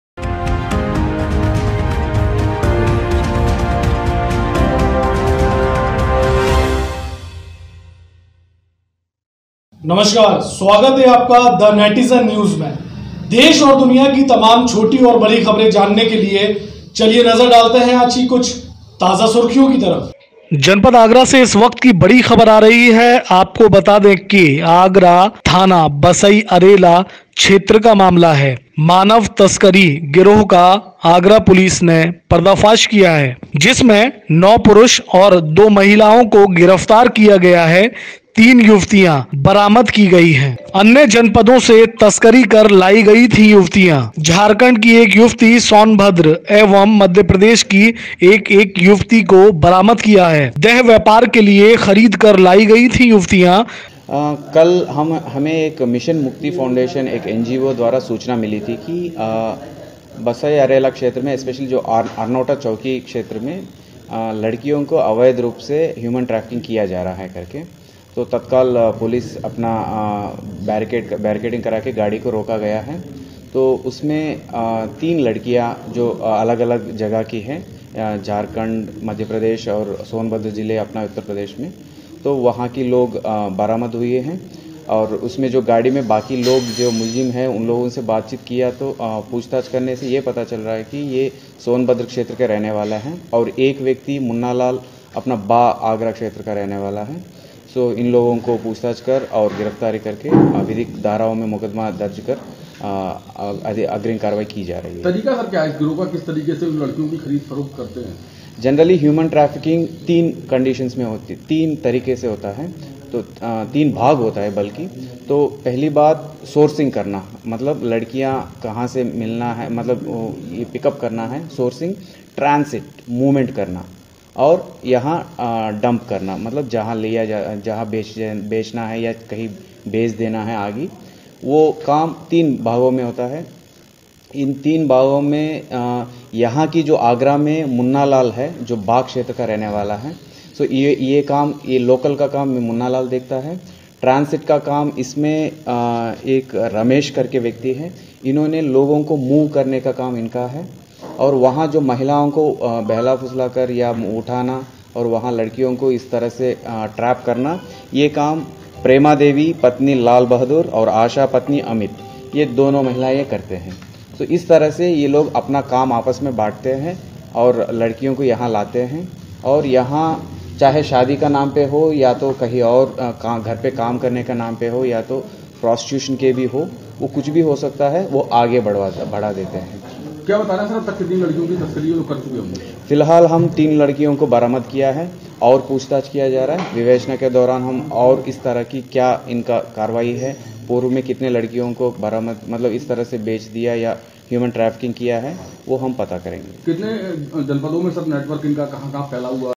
आगरा मानव तस्करी : 9 पुरुष और 2 महिलाएं गिरफ्तार
9 पुरुष और 2 महिलाएं गिरफ्तार

मानव तस्करी गिरोह का पर्दाफाश ।  9 पुरुष और 2 महिलाएं गिरफ्तार । तीन युवतियां हुई बरामद ।  मानव तस्करी की कई वारदातों का खुलासा ।

 अन्य जनपदों से तस्करी कर लाई गई थी युवतियां।  झारखंड की एक युवती बरामद । सोनभद्र की एक युवती बरामद । मध्यप्रदेश की एक युवती बरामद ।  देह व्यापार के लिए खरीद कर लाई गई थी  युवतियां । बसई अरेला थाना क्षेत्र का मामला